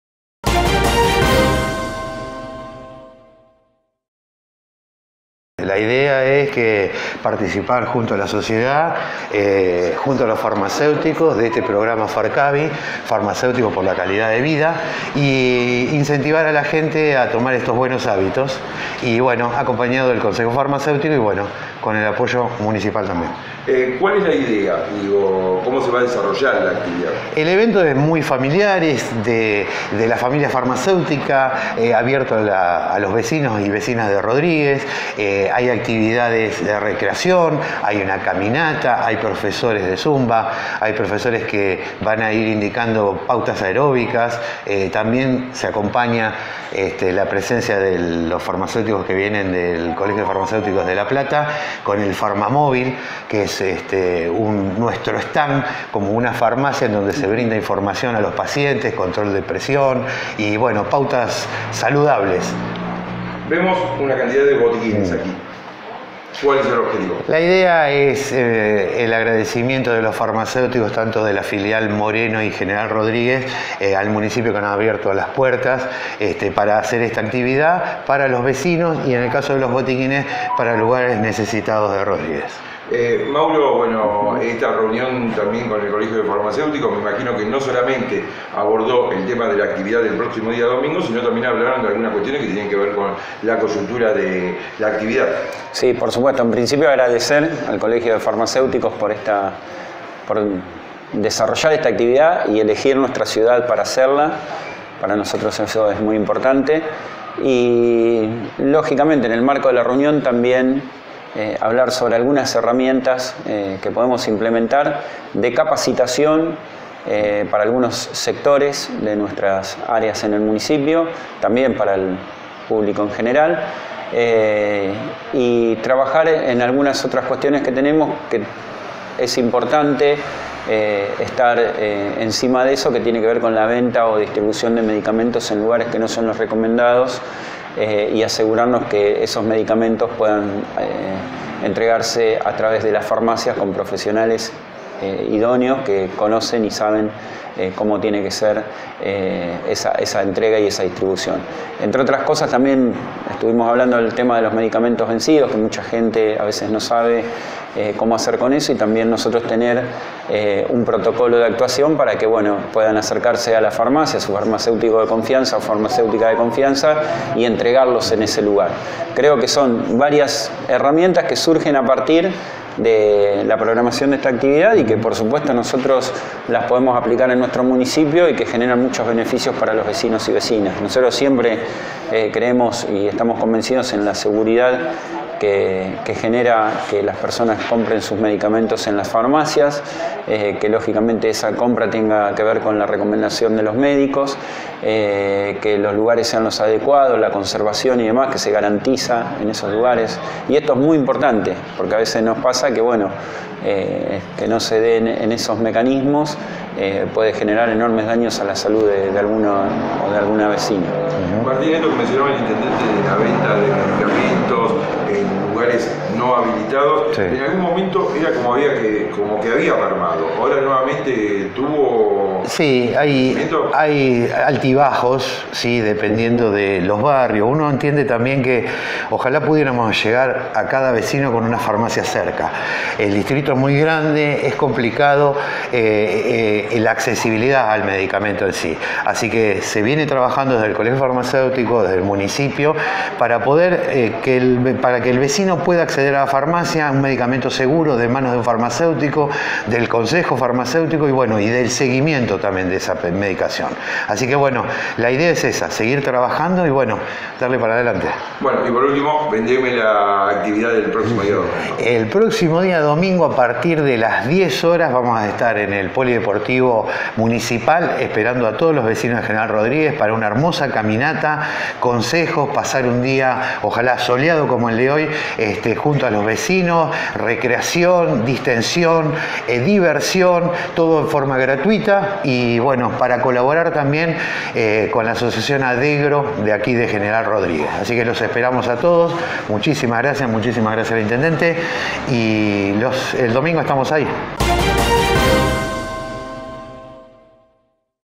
El Colegio de Farmacéuticos de la provincia avanza con la jornada deportiva en General Rodríguez
La Caminata Deportiva Solidaria se realizará de manera gratuita en el polideportivo "Juan Antonio Ávila" de la municipalidad de General Rodríguez. Habrá patio gastronómico a beneficio de ADDEGRO, entre muchas otras actividades. Entrevista con Mauro García (Intendente municipal de General Rodríguez) y Martín Aste (presidente del Colegio de Farmacéuticos de General Rodríguez)